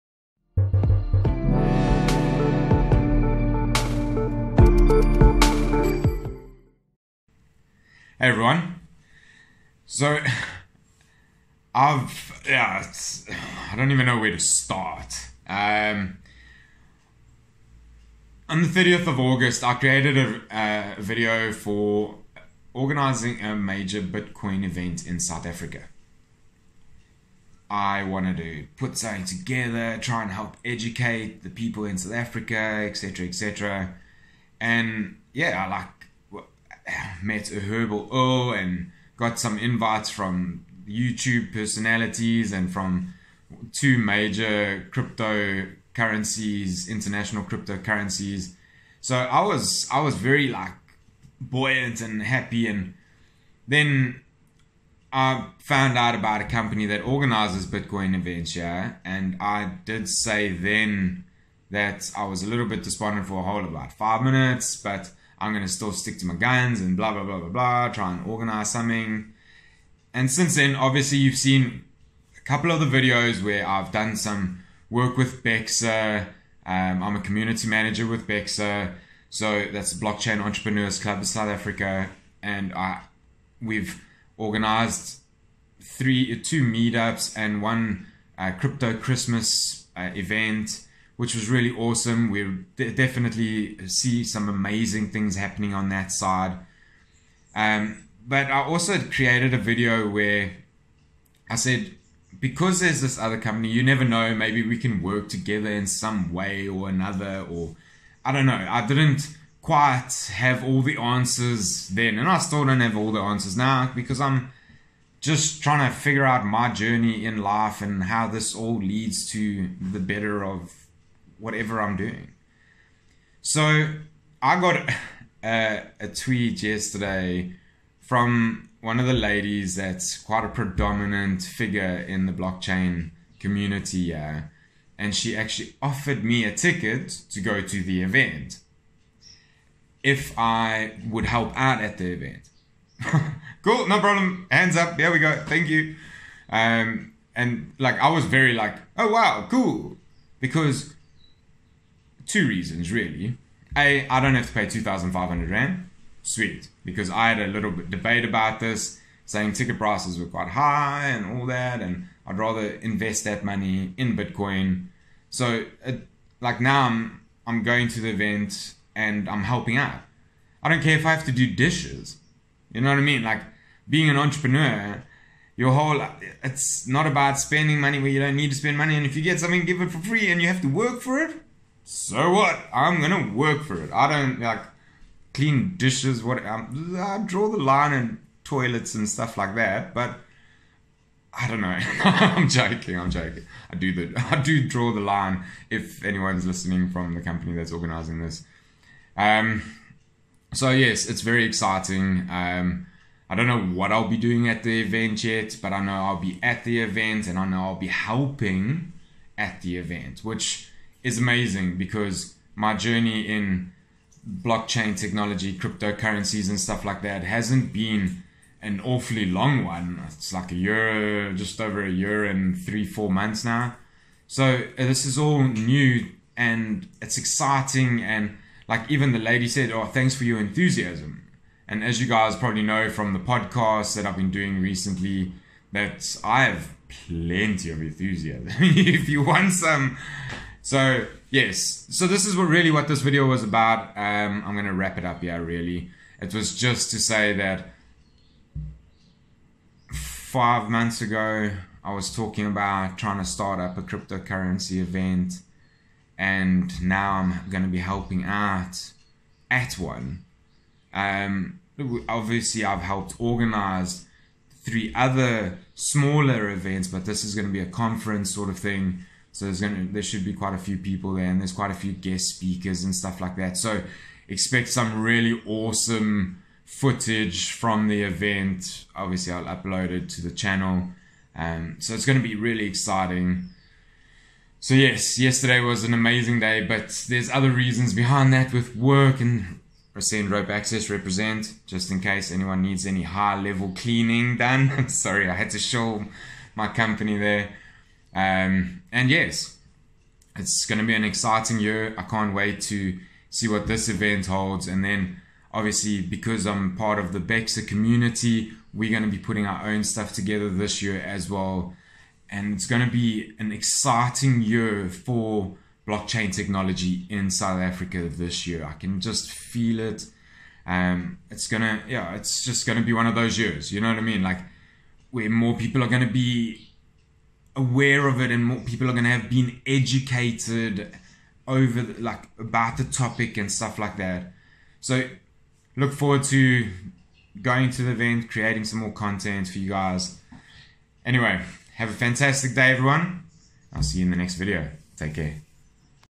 Being Part Of A Major Blockchain Event
If you have a passion you should follow it...

August last year I created a video about how I wanted to start a major Bitcoin event in South Africa.

That was the goal and the dream anyway, well I admittedly didn't have all the answers at the time but I knew this is something I wanted to be apart of...

Since then I have joined BECSA (blockchain entrepreneurs club South Africa) as a community manager for Cape Town. We have so far put together two meetups and one event and I can see so much more happening on that front.

I have now been asked if I would like to help out at a Bitcoin conference I'm not sure what I have to do yet but I don't really care, I don't have to pay for a ticket so I'm happy.

This is all about gaining more knowledge and helping out where I can...